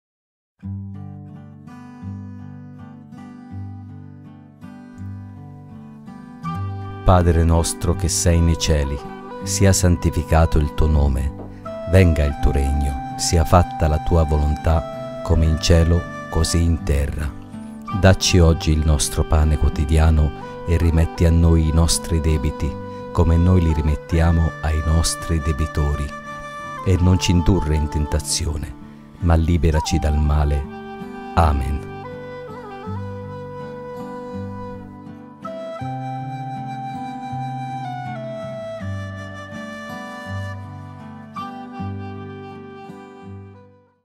PADRE NOSTRO (detto anche Preghiera del Signore)
Voi dunque pregate così: Padre nostro che sei nei cieli,  sia santificato il tuo nome;  venga il tuo regno; sia fatta la tua volontà, come in cielo così in terra.  Dacci oggi il nostro pane quotidiano, e rimetti a noi i nostri debiti  come noi li rimettiamo ai nostri debitori, e non ci indurre in tentazione, ma liberaci dal male.

Matteo 6:9-13 (CEI 1974)